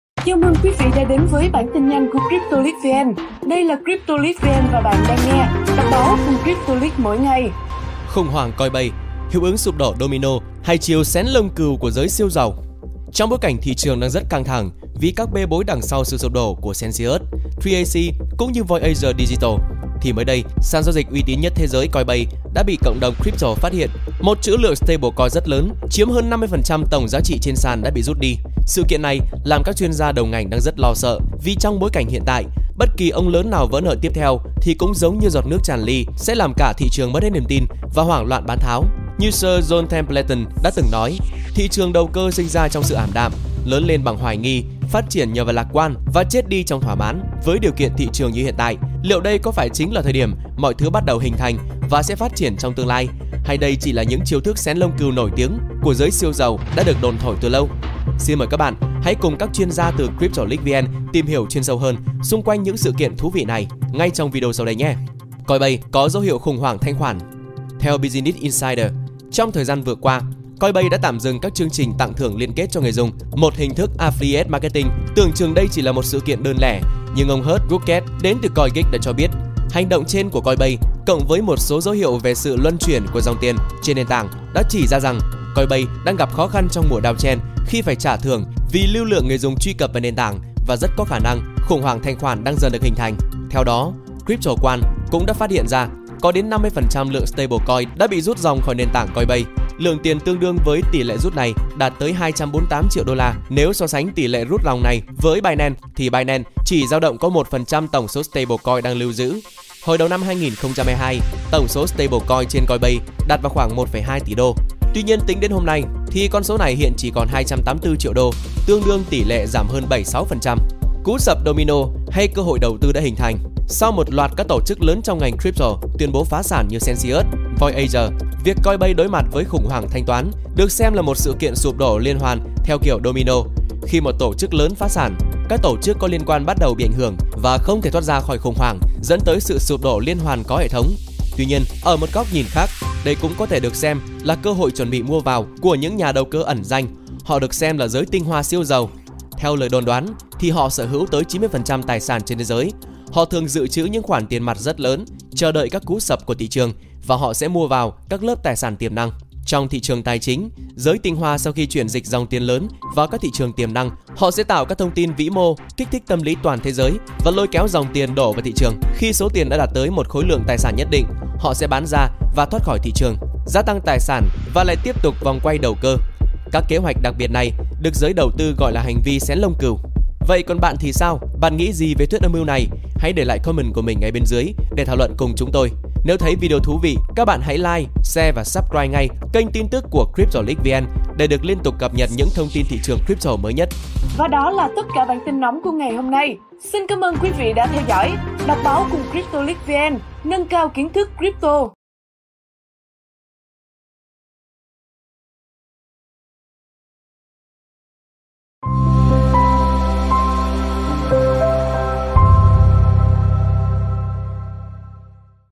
Khủng Hoảng Coinbase – Hiệu Ứng Sụp Đổ Domino Hay Chiêu “Xén Lông Cừu” Của Giới Siêu Giàu
Trong bối cảnh thị trường đang rất căng thẳngq vì các bê bối đằng sau sự sụp đỗ của Celsius, 3AC cũng như Voyager Digital. Thì mới đây, sàn giao dịch uy tín nhất thế giới – Coinbase – đã bị cộng đồng Crypto phát hiện: một trữ lượng stablecoin rất lớn – chiếm hơn 50% tổng giá trị trên sàn đã bị rút đi.
Sự kiện này làm các chuyên gia đầu ngành đang rất lo sợ, vì trong bối cảnh hiện tại, bất kỳ “ông lớn” nào vỡ nợ tiếp theo, thì cũng giống như “giọt nước tràn ly”. Sẽ làm cả thị trường mất hết niềm tin và hoảng loạn bán tháo.
Như Sir John Templeton đã từng nói: “thị trường đầu cơ sinh ra trong sự ảm đạm, lớn lên bằng hoài nghi, phát triển nhờ vào lạc quan và chết đi trong thoả mãn”.
Vậy với điều kiện thị trường hiện tại, liệu đây có phải chính là thời điểm mọi thứ bắt đầu hình thành và sẽ phát triển trong tương lai, hay đây chỉ là những “chiêu thức” xén lông cừu nổi tiếng của giới siêu giàu đã được “đồn thổi” từ lâu.
Xin mời các bạn, hãy cùng các chuyên gia từ CryptoLeakVN, tìm hiểu chuyên sâu hơn, xung quanh những sự kiện thú vị này, ngay trong video sau đây nhé.